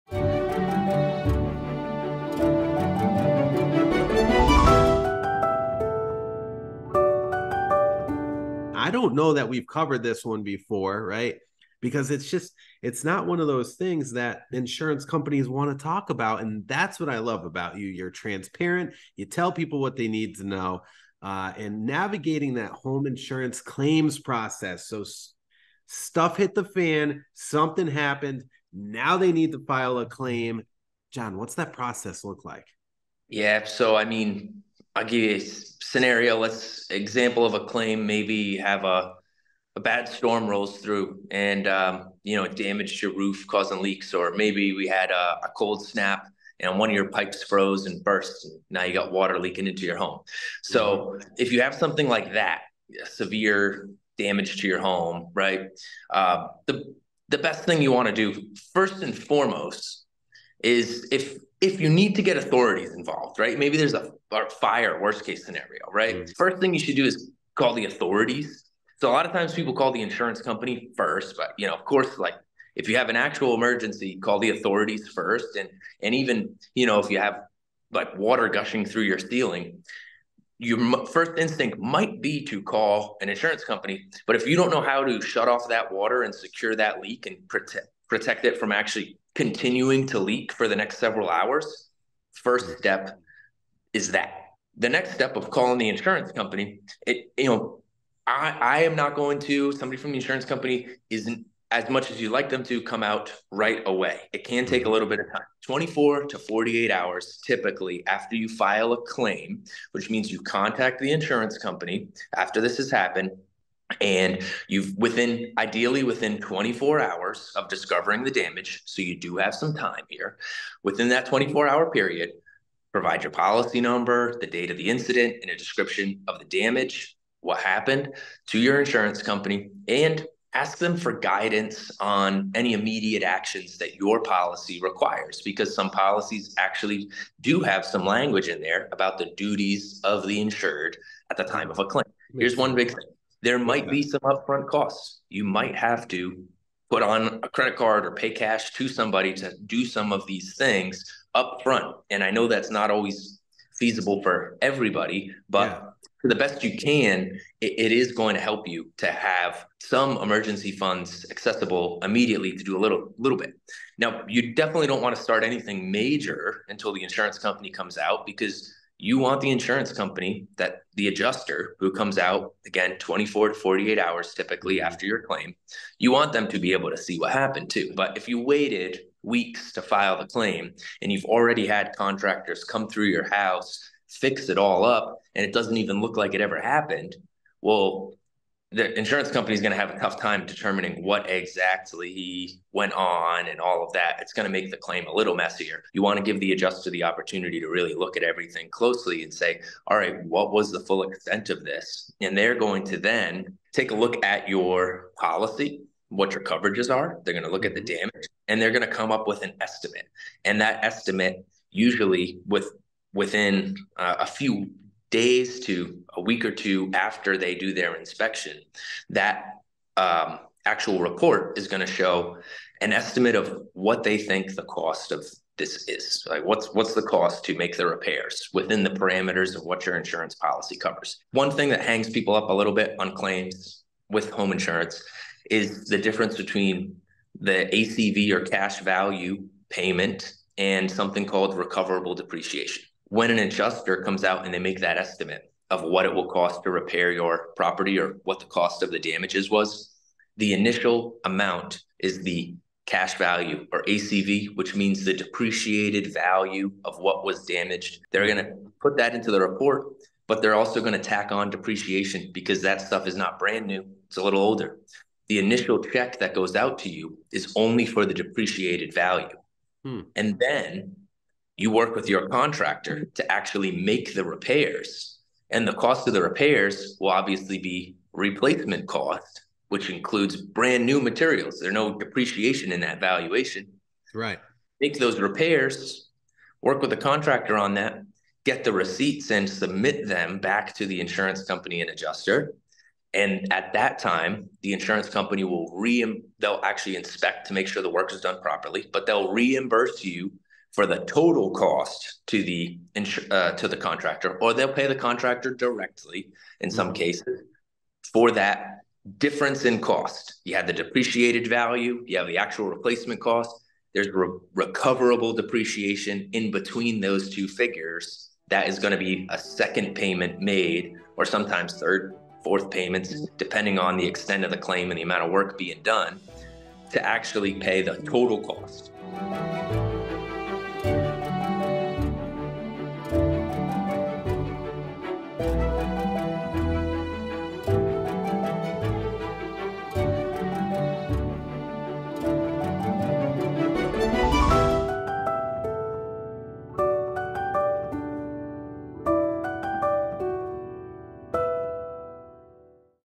Navigating The Home Insurance Claims Process
John Lapointe from Lapointe Insurance talks about how to file a claim and what to expect during the claim process.